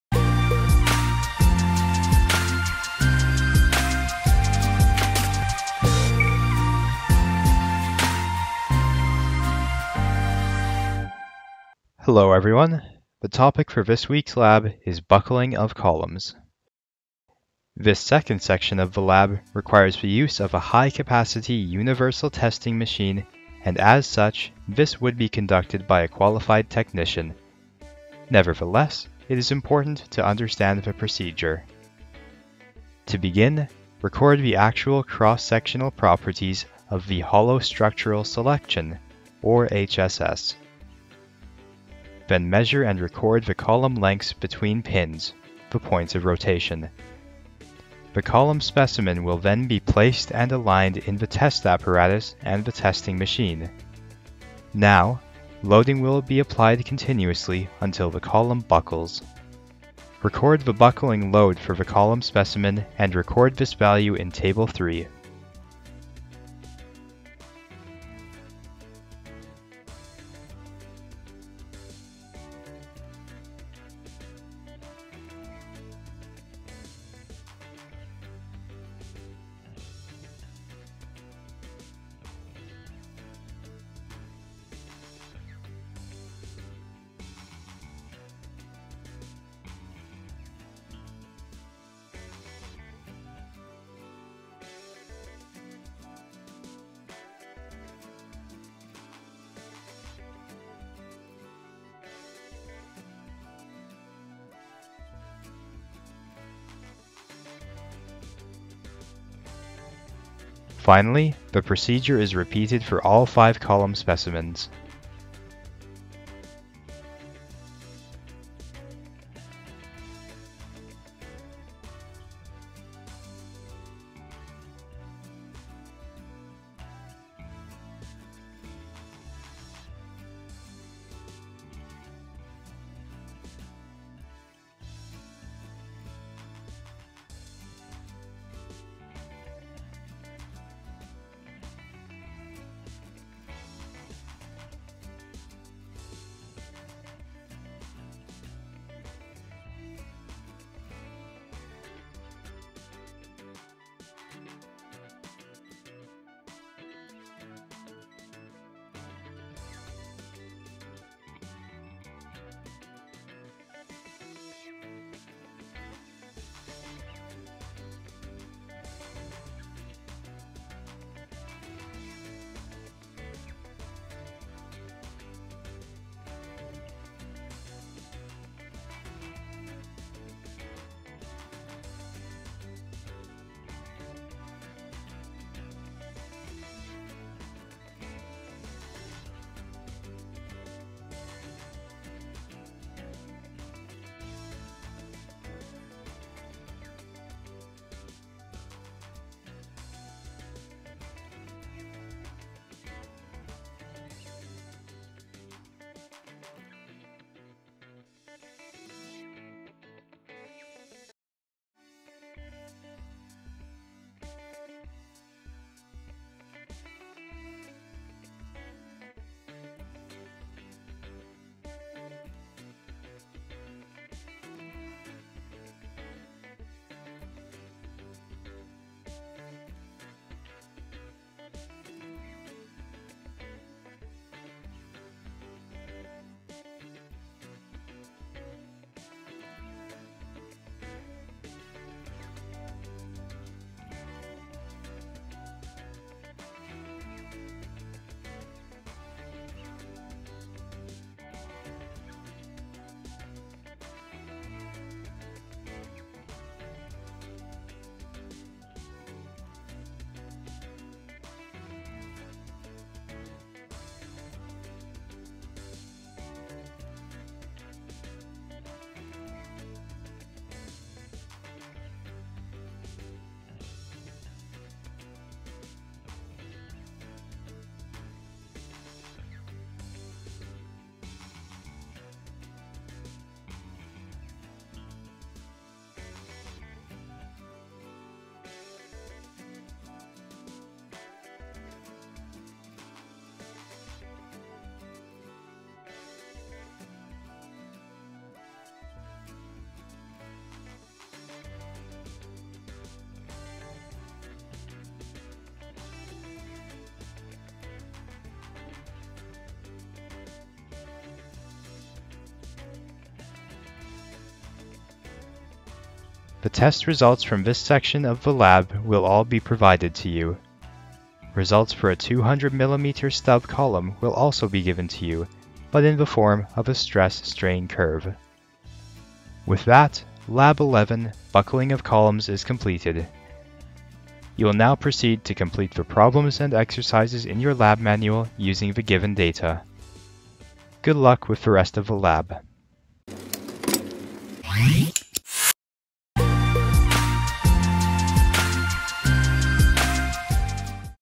CIV E270 Buckling of Columns Part 2 (Lab 11)
Course Instructors: Dr. Vivek Bindiganavile, Dr. Mustafa Gul, Dr. Mohammadali Sepehri
Lab Instructor: Mr. Greg Miller
Screenwriter: Dr. Mohammadali Sepehri
Production: Cheng-Hsuan (Jason) Yang, Weichen Qui, Matthew Blain